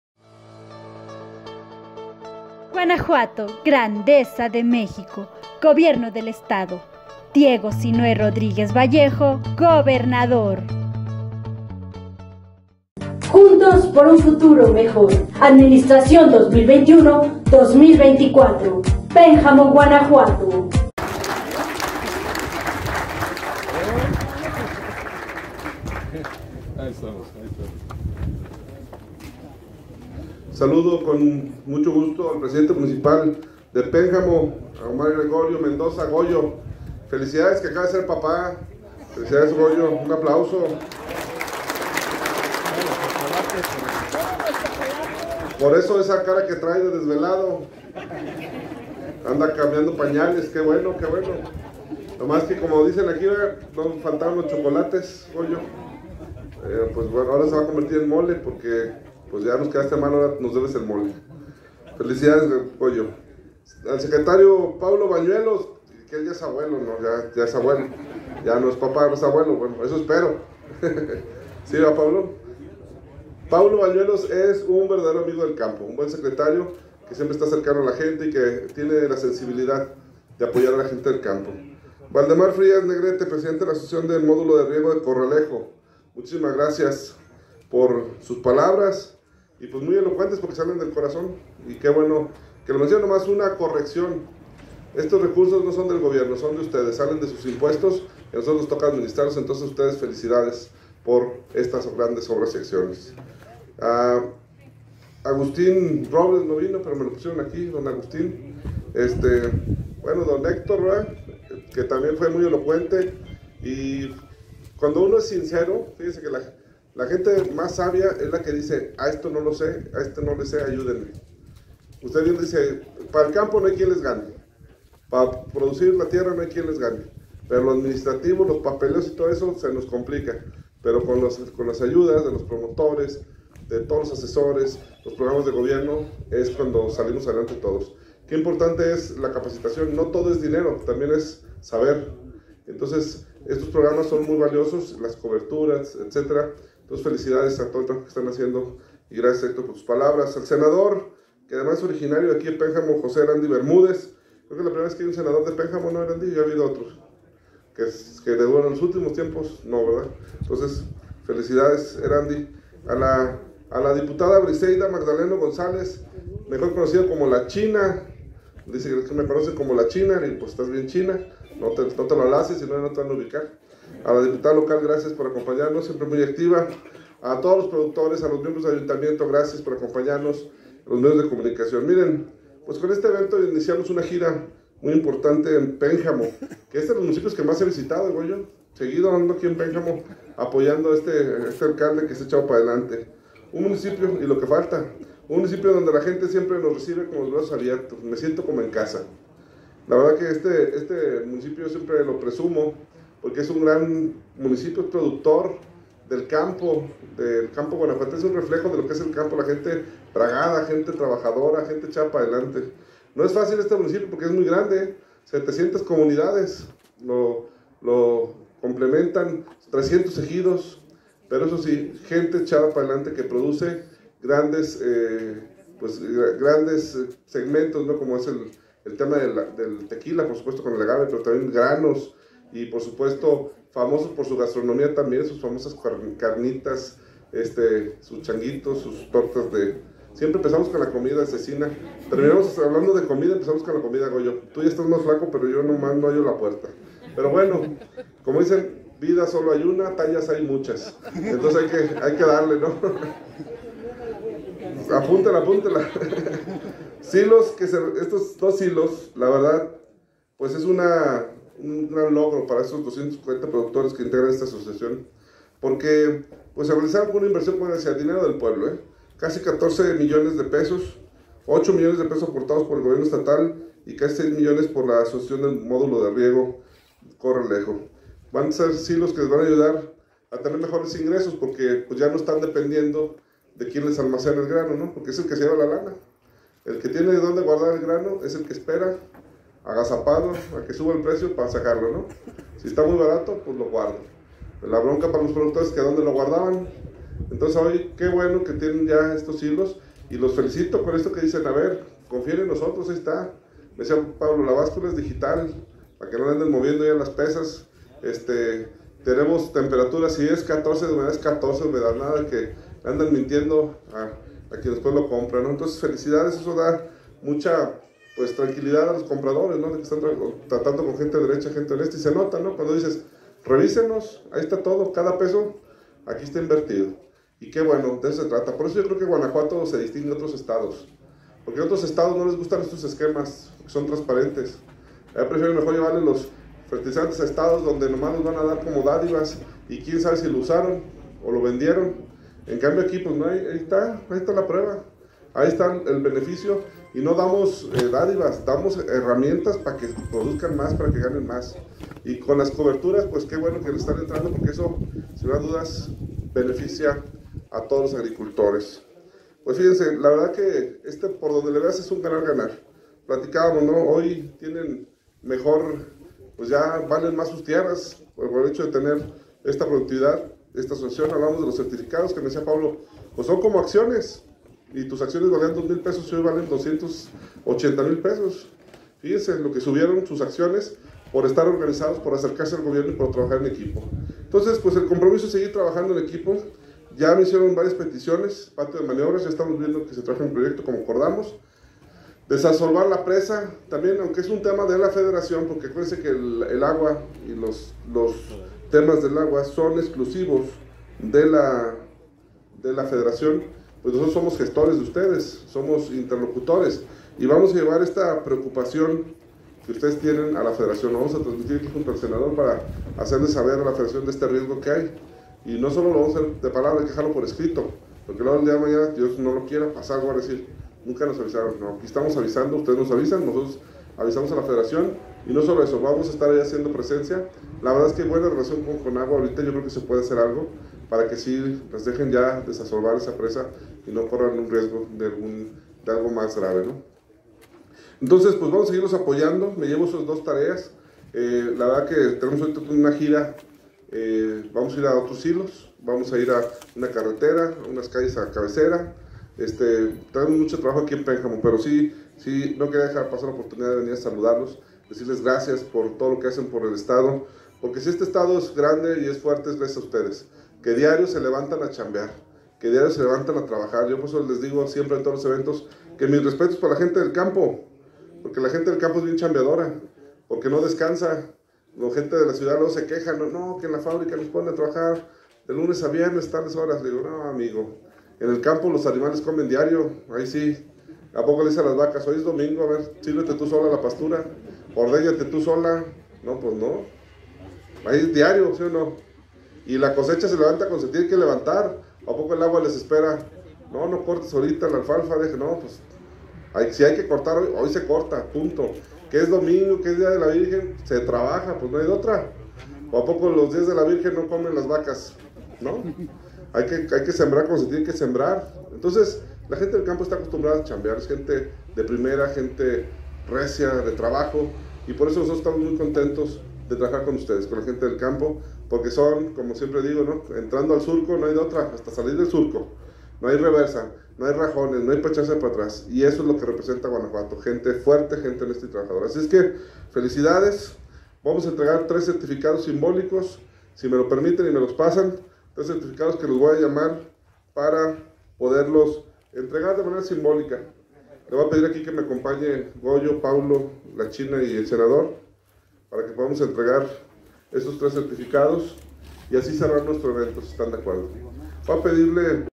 DIEGO SINÚE RODRÍGUEZ VALLEJO ENTREGA APOYOS EN PÉNJAMO.
FORTALECIENDO AL CAMPO PENJAMENSE, ACOMPAÑADO DE GOYO MENDOZA, ALCALDE DE PÉNJAMO.